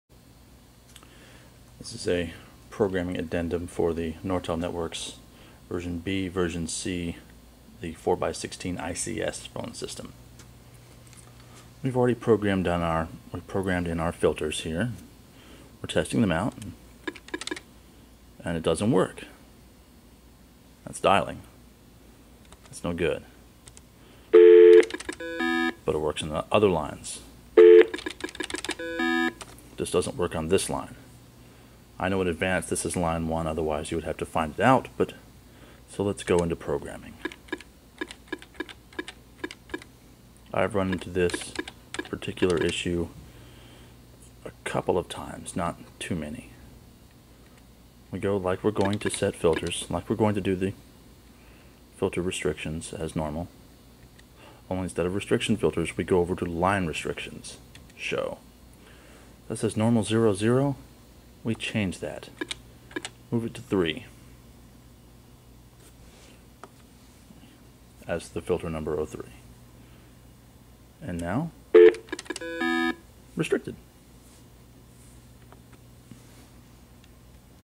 Nortel ICS sidenote.MPG
How to program a standard Nortel Networks 3x8 system to block 411 and 5551212 information calls. This refers specifically to an issue where to filter properly you need to assign the filters to lines in a different way.